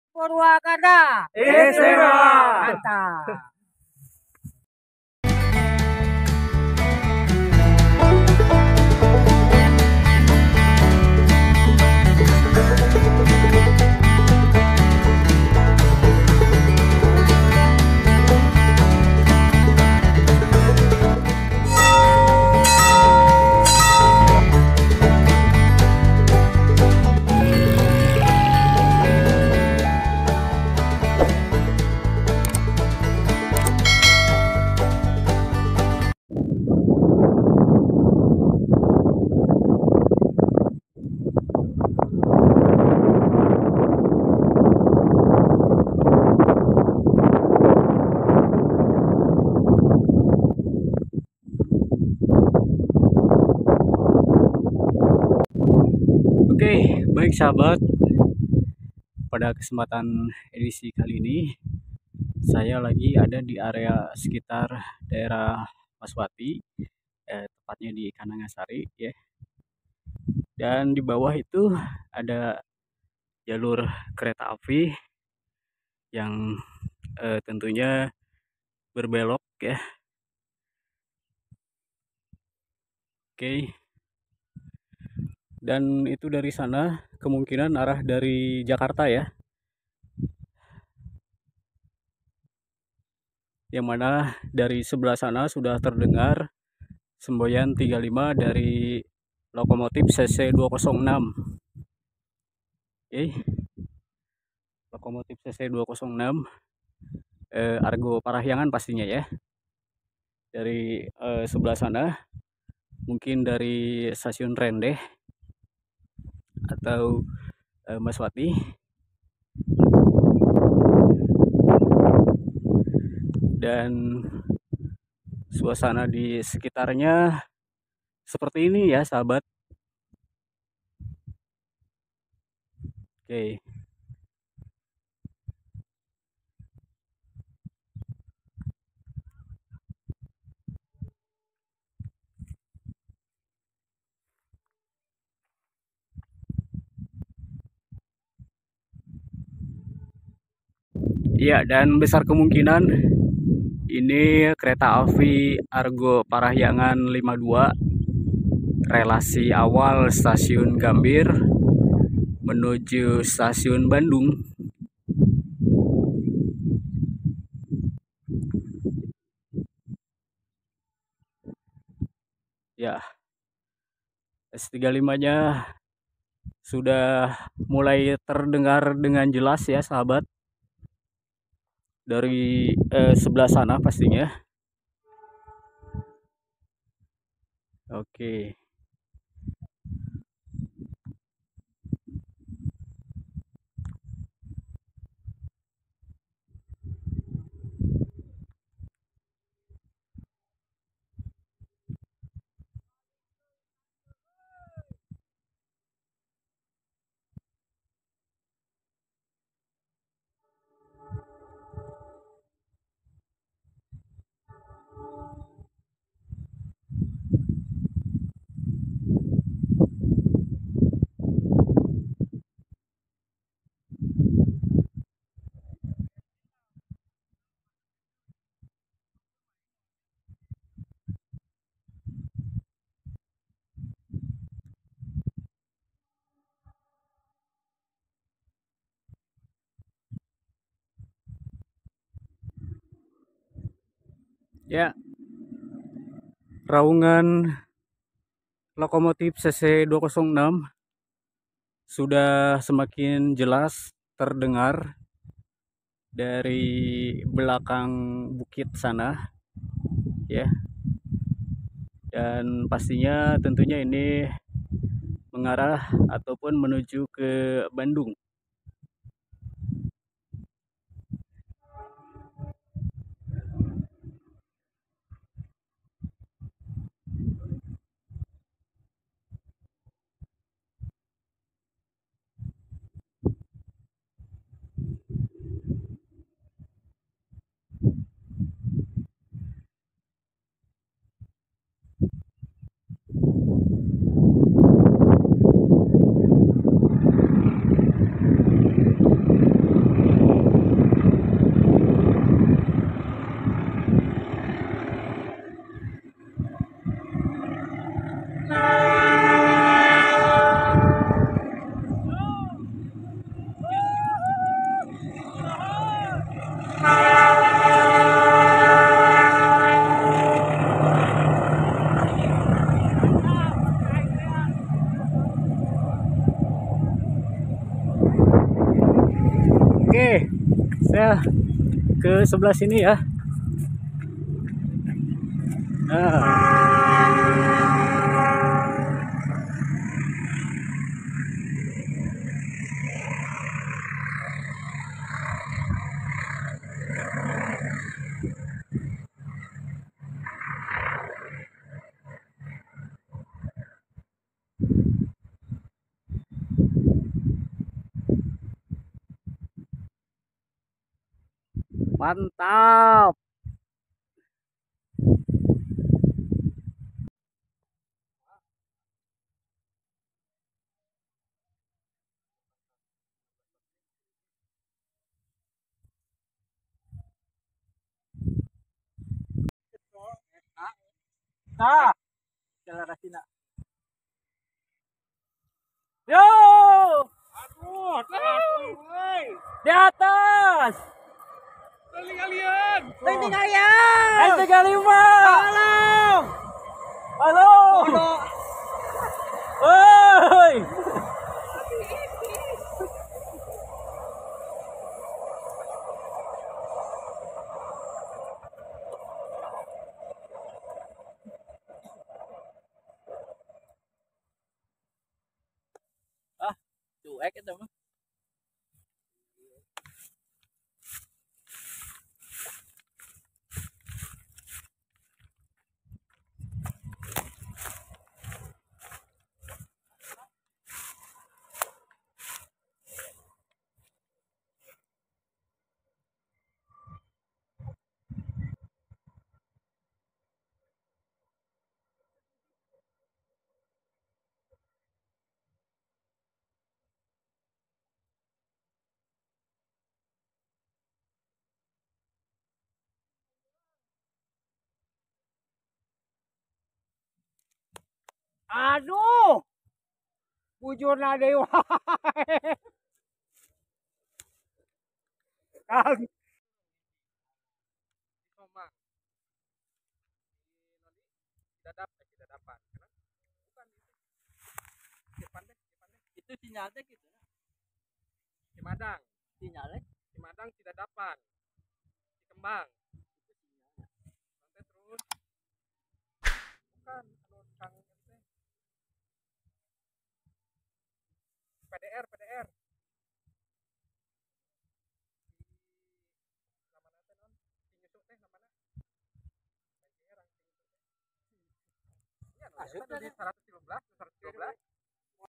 MELIHAT KERETA API ARGO PARAHYANGAN SAAT MELEWATI JALUR EKSTRIM
Ular Besi Kereta Api Argo Parahyangan Saat Melewati Jalur Ekstrim Maswati Kabupaten Bandung Barat
Hunting Kereta Api Bersama
Arif Lukman
Akmal Helmi